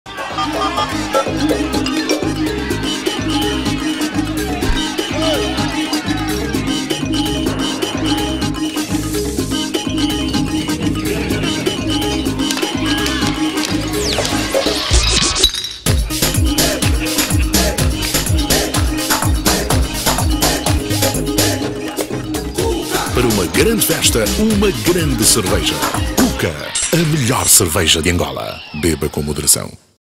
CUCA É GRANDE
Spot Cerveja Cuca "Grande Festa" - 2010
Aqui podemos ver imagens do emblemático Prédio da Cuca com o sinal luminoso no topo, já retirado actualmente.